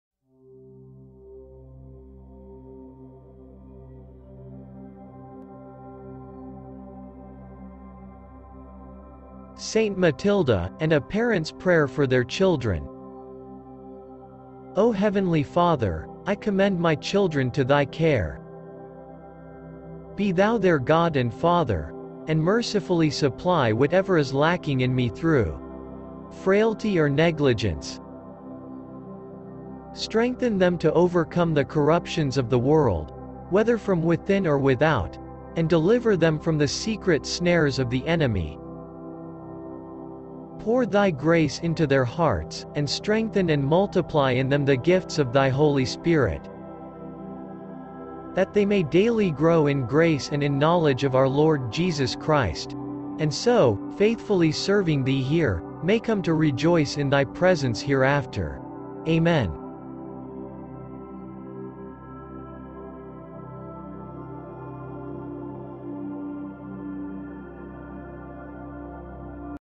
ST. MATILDA and a Parent's Prayer for their Children / Feast Day: March 14
O Heavenly Father, I commend my
children to Thy care.

Be Thou their God
and Father; and mercifully supply
whatever is lacking in me through
frailty or negligence.

 Strengthen them
to overcome the corruptions of the
world, whether from within or without; and deliver them from the secret snares of the enemy.

Pour Thy grace into their
hearts, and strengthen and multiply in them the gifts of Thy Holy Spirit.

 that they may daily grow in grace and in knowledge of our Lord Jesus Christ; and so, faithfully serving Thee here, may come to rejoice in Thy presencehereafter. Amen."

 MORE ABOUT ST. MATILDA

St. Matilda was a holy woman who lived in the 800s-900s. She was a queen, and one of her sons restored the Holy Roman Empire. She was considered by others to be very pious and holy. During her life, she established many convents.

St. Matilda was born around the year 892 in Saxony. Her father was a count. She had three sisters, and she was raised by her grandmother in an abbey. Little else is known for certain about her childhood.

In 909, Matilda married Henry, the Duke of Saxony. He later became the king of the Kingdom of the East Franks. Matilda and Henry had five children together.

Their eldest son Otto was eventually crowned the Holy Roman Emperor. Their other two sons were dukes, and one was made an archbishop. Their daughters both married dukes, and one of the two daughters married King Louis IV of France in a second marriage.

Throughout her time as queen of the Kingdom of the East Franks, Matilda established many convents in her country. She also influenced her husband’s rule for the better, helping him to rule justly. She became known for her charitable works, and those who knew her considered her to be a model of piety.

In 936, Matilda’s husband Henry died. He was buried in Quedlinburg. The same year, Matilda established a convent in Quedlinburg.

Now that the throne of Matilda’s husband was vacant, she supported the claim of her son Henry to the throne. Otto was elected to the throne, and Henry led a revolt which was not successful. Matilda then convinced Otto to name Henry the Duke of Bavaria.

Matilda soon began living in the convent in Quedlinburg. The Quedlinburg Abbey eventually became a very significant place for prayer and commemoration of the dead in the Kingdom of the East Franks.

Matilda died in 968 after a lengthy illness.

Hello everyone, Peace be with you. Today i made this content for the FEAST OF ST. MATILDA which is on March 14.  Patron Saint of Parents and of Large Family. You can make this Prayer anytime you want for your own intentions.

❣❣❣❣ Credits / Sources: ❣❣❣❣

John 11:40